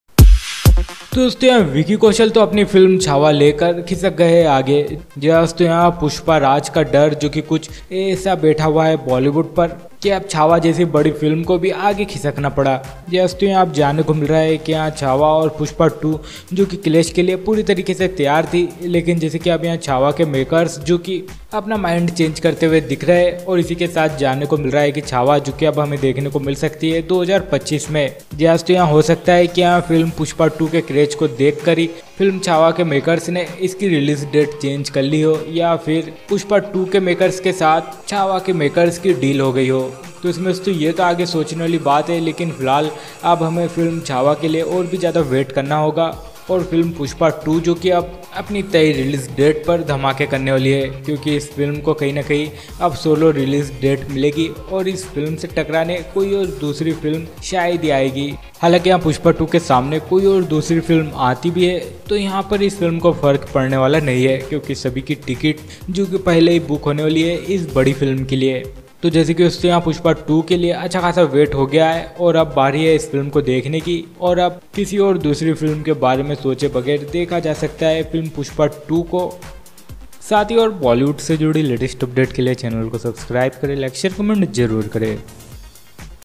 Vicky Kaushal's Chhaava Postponed Due To Allu Arjun's Pushpa 2 Fear
Bollywood Latest Entertaining News & Update